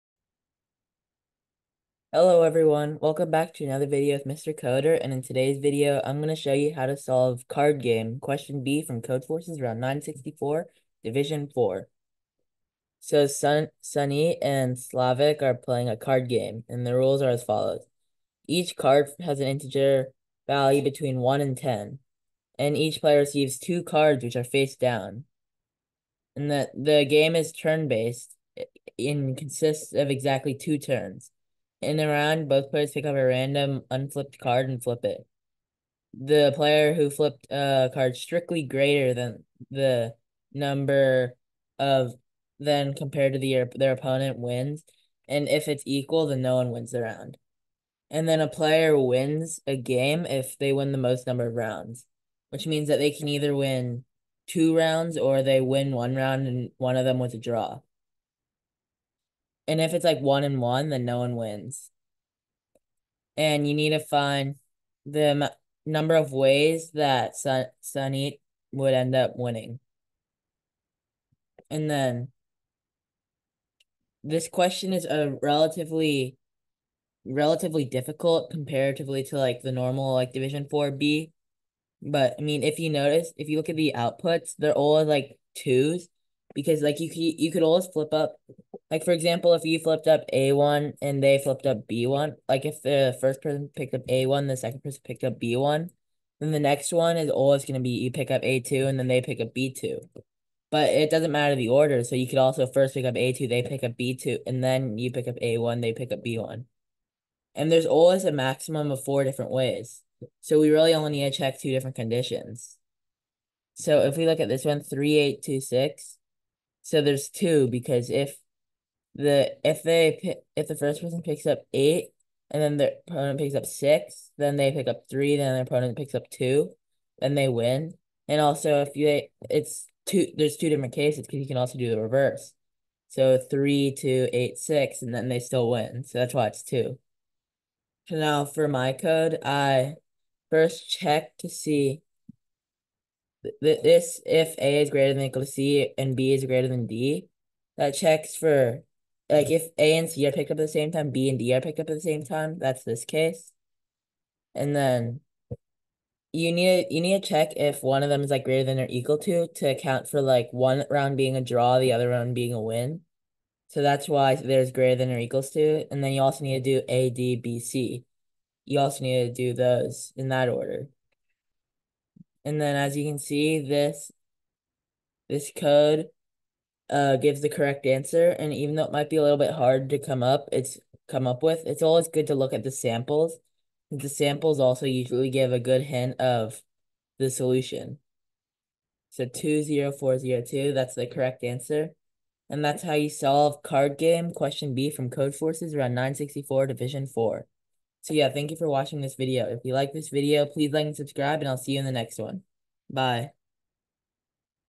Card Game - Codeforces Round 964 (Div. 4) Problem B Solution
This video shows you how to solve Card Game, Problem B from Codeforces Round 964 (Div. 4).